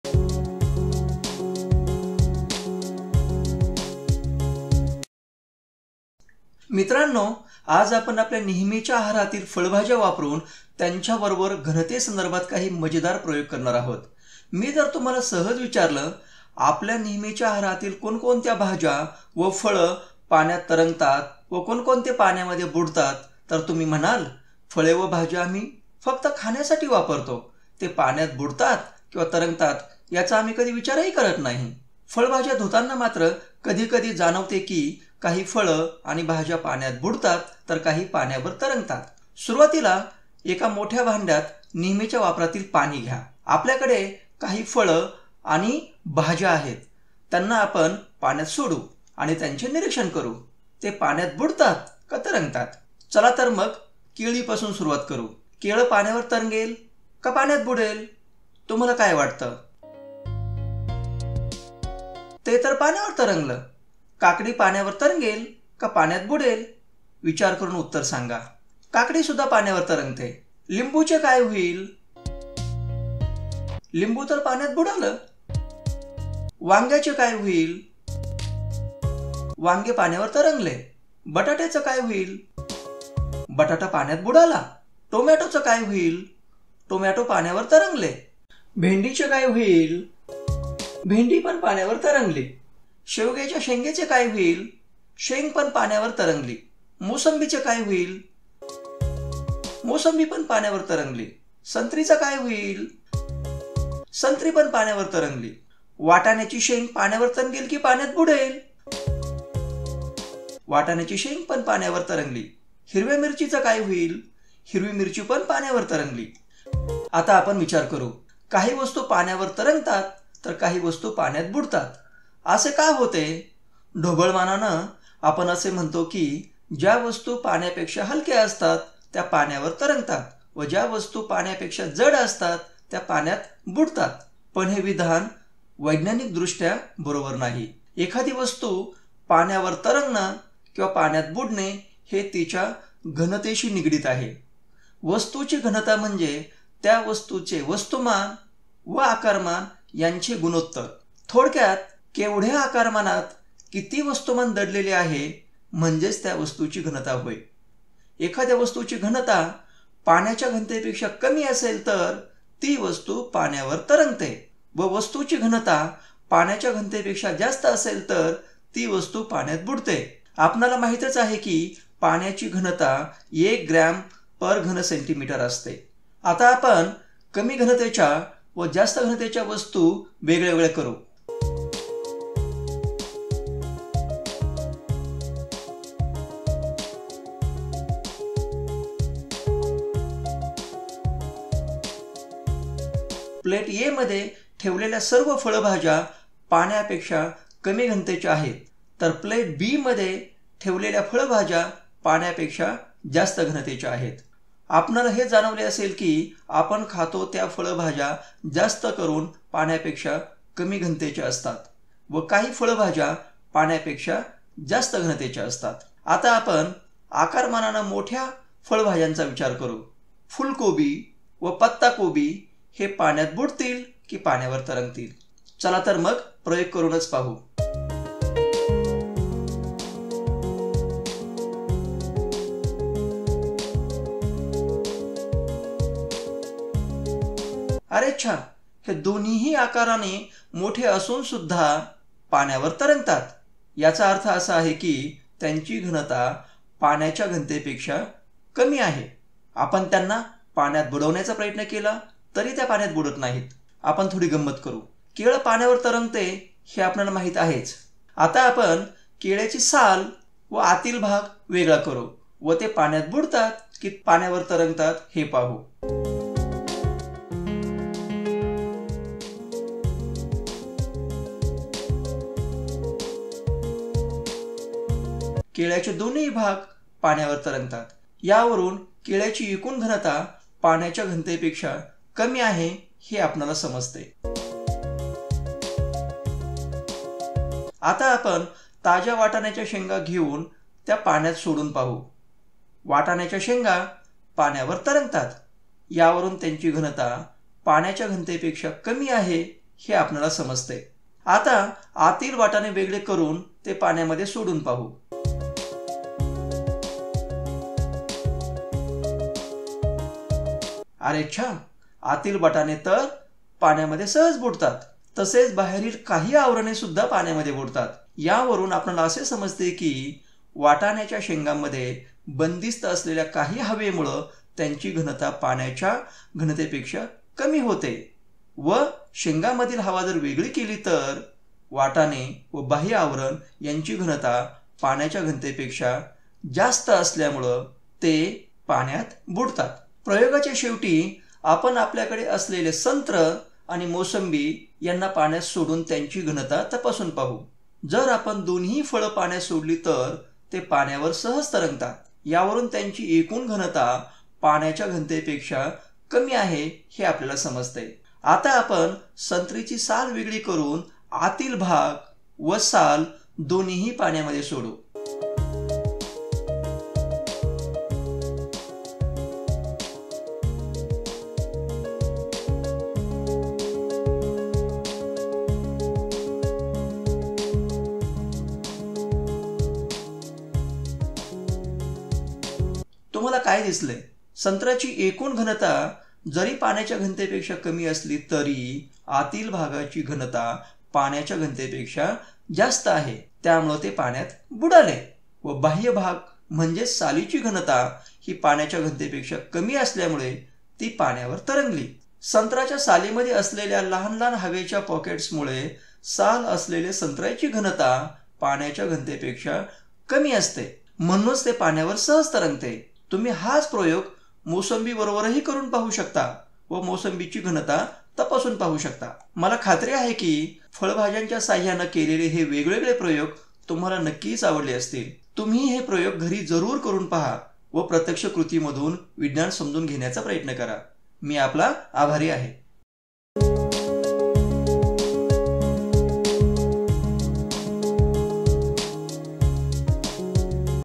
Why do some vegetables sink or float? | Marathi
In this video, we are explaining how things float in the water and how things sink in the water.

मित्रांनो आज आपण आपल्या नेहमीच्या आहारातील फळभाज्या वापरुन यांच्याबरोबर घनतेसंदर्भात काही मजेदार प्रयोग करणार आहोत. मी जर तुम्हाला सहज विचारले. आपल्या नेहमीच्या आहारातील कोणकोणत्या भाज्या व फळे पाण्यात तरंगतात व कोणकोणते पाण्यात बुडतात तर तुम्ही म्हणाल फळे व भाज्या आम्ही फक्त खाण्यासाठी वापरतो ते पाण्यात बुडतात किंवा तरंगतात याचा आम्ही कधी विचारच करत नाही. फळभाज्या धुतांना मात्र कधी कधी जाणवते कि काही फळं आणि भाज्या पाण्यात बुडतात तर काही पाण्यावर तरंगतात. सुरवातीला एका मोठ्या भांड्यात नेहमीच्या वापरातील पाणी घ्या. आपल्याकडे काही फळं आणि भाज्या आहेत त्यांना आपण पाण्यात सोडू आणि त्यांचे निरीक्षण करु, ते पाण्यात बुडत्यात का तरंगतात.  चला तर मग केळीपासुन सुरुवात करु, केळं पाण्यावर तरंगेल का पाण्यात बुडेल तुम्हाला काय वाटते. ते तर पाण्यावर तरंगले. काकडी पाण्यावर तरंगेल का पाण्यात बुडेल विचार करुन उत्तर सांगा. काकडीसुद्धा पाण्यावर तरंगते,  लिंबू चे काय होईल, लिंबू तर पाण्यामध्ये बुडाले. वांग्याचे काय होईल? वांगे पाण्यावर तरंगले. बटाट्याचे काय होईल, बटाटा पाण्यात बुडाला. टोमॅटोचे काय होईल, टोमॅटो पाण्यावर तरंगले,  शेवग्याच्या शेंगेचे काय होईल, शेंग पण पाण्यावर तरंगली. मोसंबीचे काय होईल. मोसंबी पण पाण्यावर तरंगली. संत्रीचे काय होईल. संत्री पण पाण्यावर तरंगली. वाटाण्याची शेंग पाण्यावर तरंगेल कि पाण्यात बुडेल. वाटाण्याची शेंग पण पाण्यावर तरंगली. हिरव्या मिरचीचे काय होईल. मिरची पण पाण्यावर तरंगली.
आता आपण विचार करू काही वस्तू पाण्यावर तरंगतात तर काही वस्तू पाण्यात बुडतात असे का होते? ढोबळमानाने आपण असे म्हणतो कि ज्या वस्तू पाण्यापेक्षा हलक्या असतात त्या पाण्यावर तरंगतात व ज्या वस्तू पाण्यापेक्षा जड असतात त्या पाण्यात  बुडतात. पण हे विधान वैज्ञानिक दृष्ट्या बरोबर नाही. एखादी वस्तू पाण्यावर तरंगणे किंवा पाण्यात बुडणे हे तिच्या घनतेशी निगडीत असते. वस्तूची घनता म्हणजे त्या वस्तूचे वस्तूमान व आकारमान यांचे गुणोत्तर, थोडक्यात केवढ्या आकारमानात किती वस्तुमान दडलेले आहे म्हणजेच त्या वस्तूची घनता होय. एखाद्या वस्तूची घनता पाण्याच्या घनतेपेक्षा कमी असेल तर ती वस्तू पाण्यावर तरंगते व वस्तूची घनता पाण्याच्या घनतेपेक्षा जास्त असेल तर ती वस्तू पाण्यात बुडते. आपणाला माहीतच आहे कि पाण्याची घनता १ ग्रॅम/घन सेंटिमीटर आहे.

आता आपण कमी घनतेच्या व जास्त घनतेच्या वस्तू वेगवेगळ्या करु. प्लेट “A” मध्ये ठेवलेल्या सर्व फळभाज्या पाण्यापेक्षा कमी घनतेच्या आहेत तर प्लेट "B" मध्ये ठेवलेल्या फळभाज्या पाण्यापेक्षा जास्त घनतेच्या आहेत. आपणाला हे जाणवले असेल कि आपण खातो त्या फळभाज्या जास्तकरुन पाण्यापेक्षा कमी घनतेच्या असतात व काही फळभाज्या मात्र पाण्यापेक्षा जास्त घनतेच्या असतात.
आता आपण आकारमानानें मोठ्या फळभाज्यांचा विचार करू. फुलकोबी व पत्ता कोबी हे पाण्यात बुडतील कि पाण्यावर तरंगतील, चला तर मग प्रयोग करुनच पाहू. अरेच्या हे दोन्हींनी आकाराने मोठे असूनसुद्धा पाण्यावर तरंगतात याचा अर्थ असा आहे हि त्यांची घनता पाण्याच्या घनतेपेक्षा कमी आहे. आपण त्यांना पाण्यात बुडविण्याचा प्रयत्न केला तरी त्या पाण्यात बुडत नाहीत.
आपण थोडी गंमत करु. केळं पाण्यावर तरंगते हे आपणाला माहीतच आहे. आता आपण केळ्याची साल व आतील भाग वेगळा करू व ते पाण्यात बुडतात कि पाण्यावर तरंगतात हे पाहू. केळ्याचे दोन्हींनी भाग पाण्यावर तरंगतात. यावरुन केळ्याची  एकूण घनता पाण्याच्या घनतेपेक्षा कमी आहे हे आपणाला समजते
आता आपण ताज्या वाटाण्याच्या शेंगा घेऊन त्या पाण्यात सोडून पाहू. वाटाण्याच्या शेंगा पाण्यावर तरंगतात यावरुन त्यांची घनता पाण्याच्या घनतेपेक्षा कमी असते हे आपणाला समजते. आता आतील वाटणे वेगळे करुन ते पाण्यामध्ये सोडून पाहू, अरेच्या आतील वाटाणे तर पाण्यामध्ये सहज बुडतात तसेच बाहेरील काही आवरणे सुद्धा पाण्यामध्ये बुडतात. यावरून आपणाला असे समजते कि वाटाण्याच्या शेंगांमध्ये बंधिस्त असलेल्या हवेमुळे त्यांची घनता पाण्यापेक्षा कमी होते व शेंगांमधील हवा वेगळी केली तर वाटाणे व बाह्य आवरण यांची घनता पाण्याच्या घनतेपेक्षा जास्त असते त्यामुळे ते पाण्यात बुडतात.
प्रयोगाच्या शेवटी आपण आपल्याकडे असलेले संत्र आणि मोसंबी यांना पाण्यात सोडून त्यांची घनता तपासून पाहू. जर आपण दोन्हीही फळं पाण्यात सोडली तर ते पाण्यावर सहज तरंगतात यावरून त्यांची एकूण घनता पाण्याच्या घनतेपेक्षा कमी आहे हे आपणाला समजते. आता आपण संत्राची साल वेगळी करुन आतील भाग व साल दोन्हीही पाण्यात सोडू. तुम्हाला काय दिसले. संत्राची एकूण घनता जरी पाण्याच्या घनतेपेक्षा कमी असली तरी आतील भागाची घनता पाण्याच्या घनतेपेक्षा जास्त आहे त्यामुळे ते पाण्यात बुडाले व बाह्य भाग म्हणजेच सालीची घनता हि पाण्याच्या घनतेपेक्षा कमी असल्यामुळे ती पाण्यावर तरंगली. संत्राच्या सालीमध्ये असलेल्या लहान-लहान हवेच्या पॉकेट्स मुळे साल असलेल्या संत्राची घनता पाण्याच्या घनतेपेक्षा कमी असते म्हणूनच ते पाण्यावर सहज तरंगते. तुम्ही हाच प्रयोग मोसंबीबरोबरही करुन पाहू शकता व मोसंबीची घनता तपासून पाहू शकता.

मला खात्री आहे कि फळभाज्यांच्या साहाय्याने केलेले हे वेगवेगळे प्रयोग तुम्हाला नक्कीच आवडले असतील. तुम्हीहि हे प्रयोग घरी जरुर करून पहा व प्रत्यक्ष कृतीतून विज्ञान समजून घेण्याचा प्रयत्न करा. मी आपला आभारी आहे.